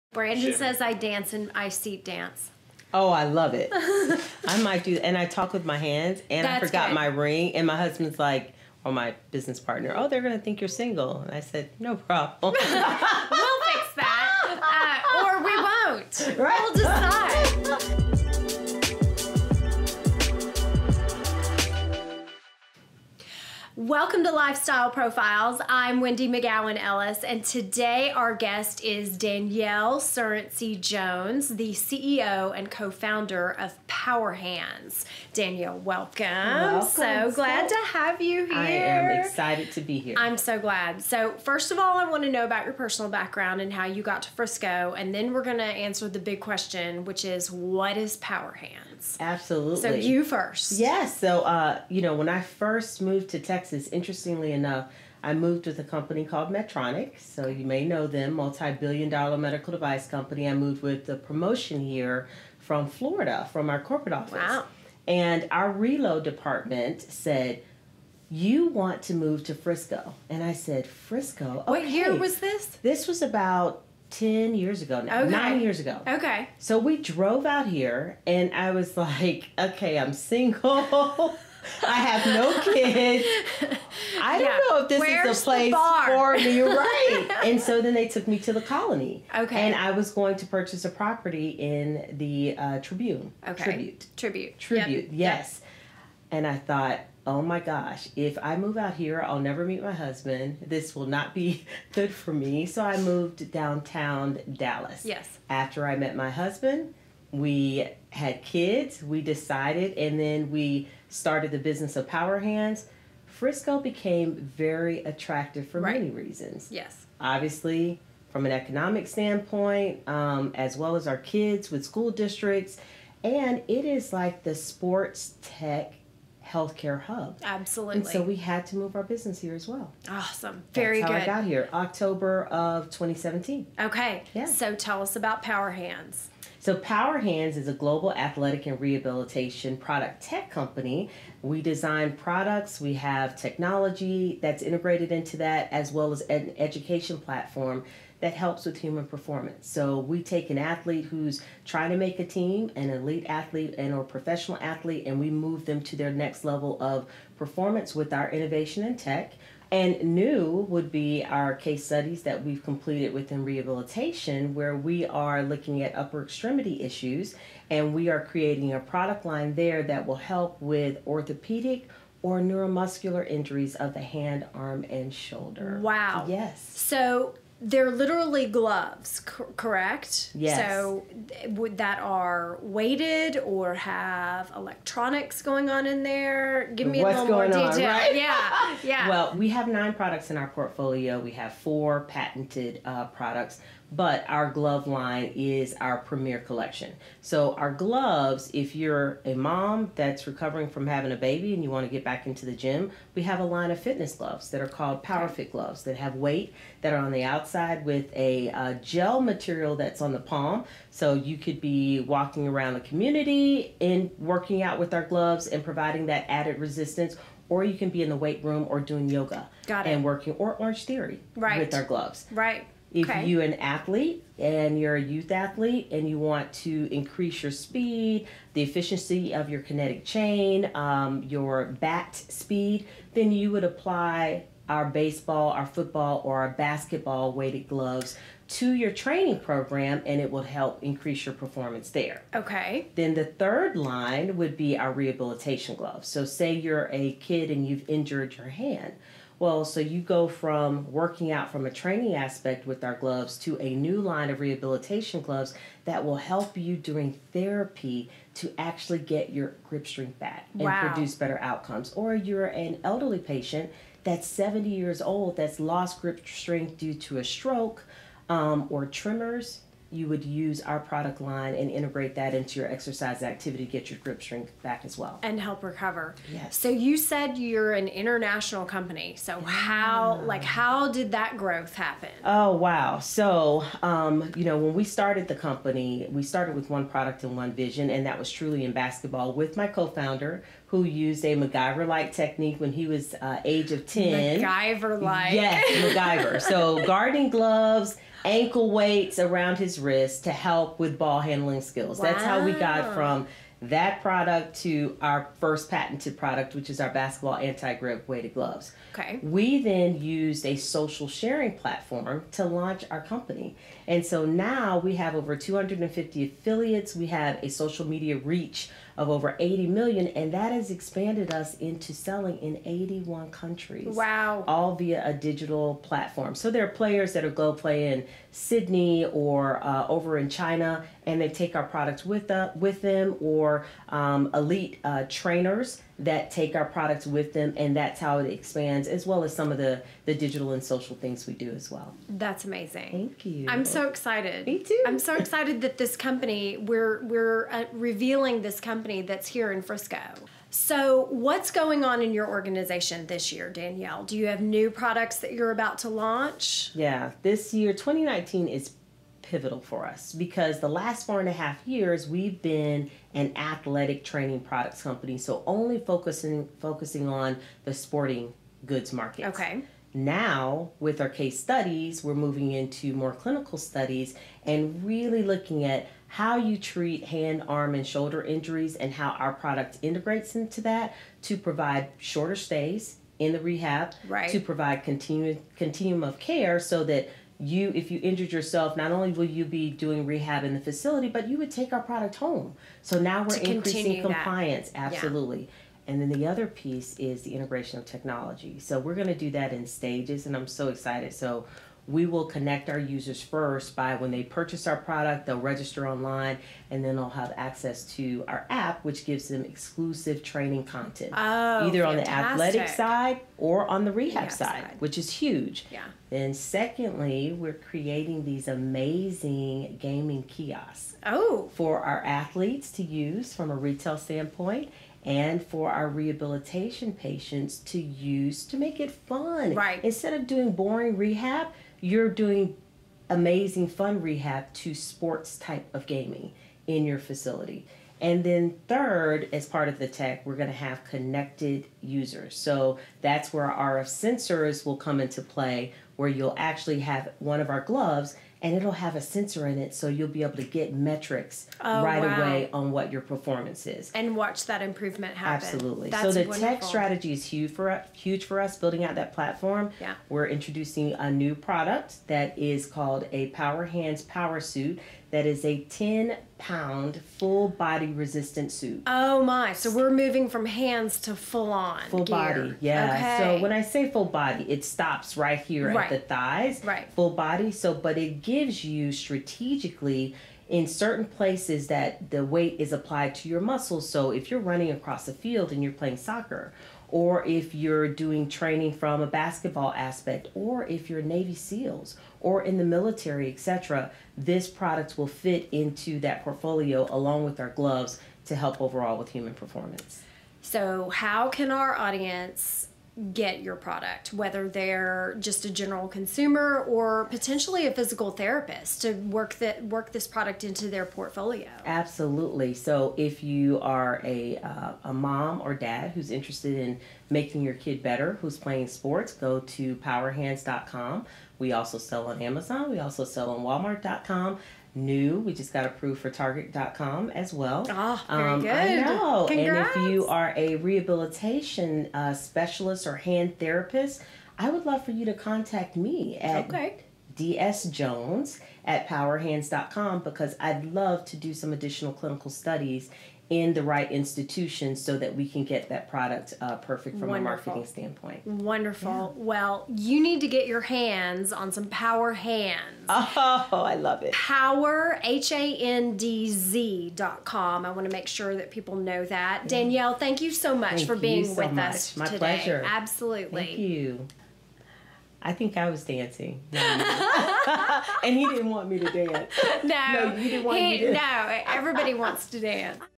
Danyel Surrency-Jones - The Power and Sophistication Behind PowerHandz
Interview #69

From working at a medical device company, to co-founding and leading a global athletic product tech company, PowerHandz, Danyel Surrency-Jones has a passion for and commitment to athletic excellence.